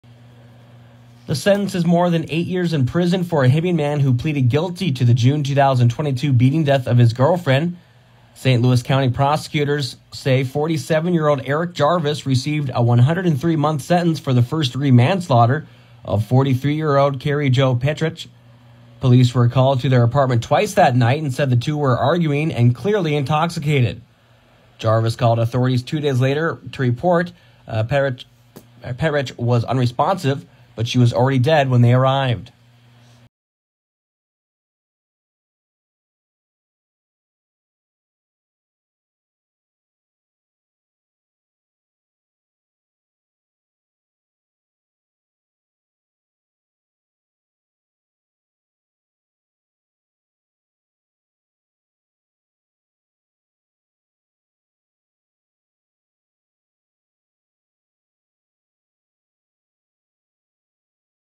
Sentenced In Beating Death Of Girlfriend
HIBBING, MN (KROXAM.com) A Hibbing, Minnesota man has been sentenced to 8 years in prison for the beating death of his girlfriend.